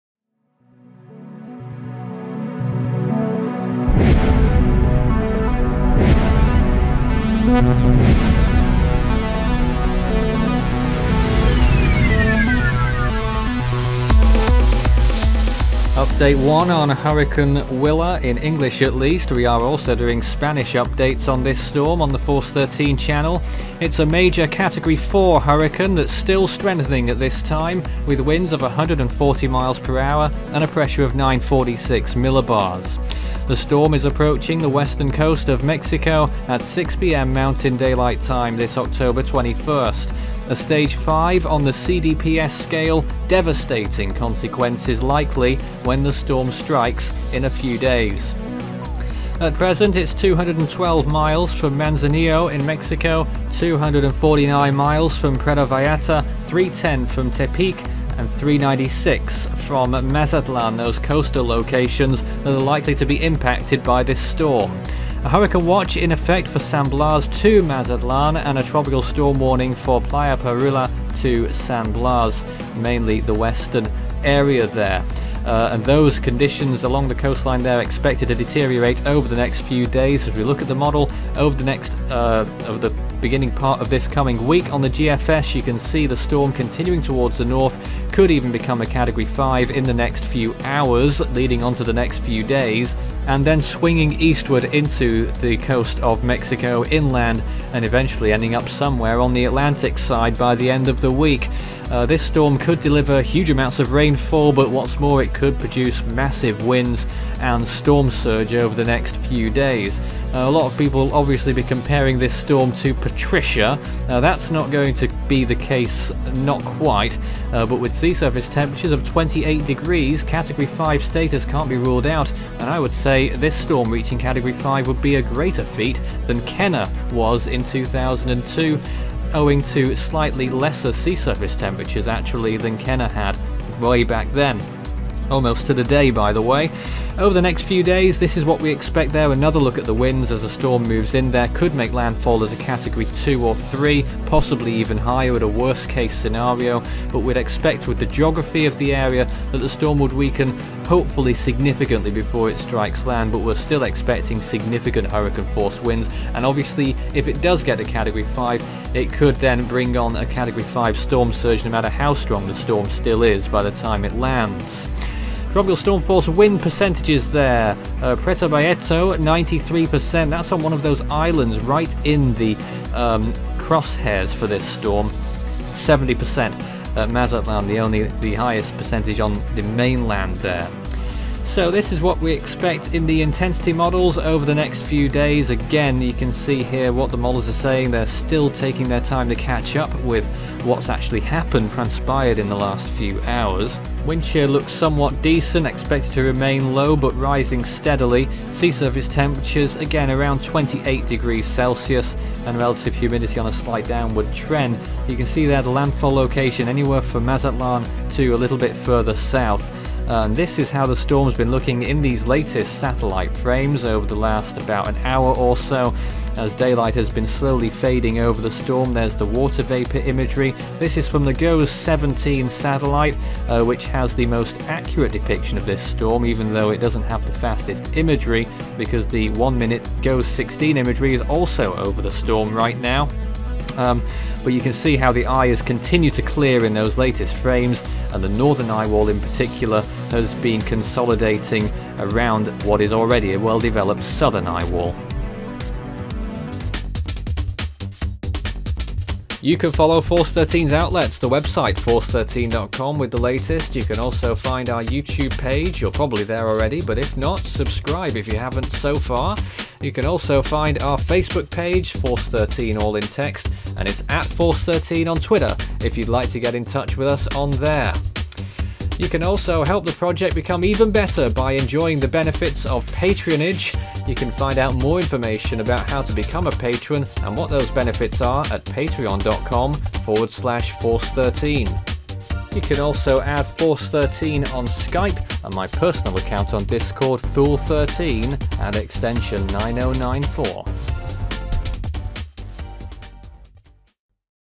Hurricane Willa explosively intensifies - 6pm MDT Oct 21, 2018
There may be small errors in the update due to using new protocols and rushing the update to completion.
Hurricane Willa continues to intensify and is now a rapidly deepening Category 4 hurricane, with the possibility for the storm reaching Category 5 status overnight. However, a Patricia scenario is deemed to be unlikely due to much cooler sea surface temperatures. Indeed, Willa would perform more of a feat than Kenna should it reach the top category, with only marginal conditions ahead of it. Land areas are also severely threatened by Willa, with a Hurricane Watch as far north as Mazatlan at this time.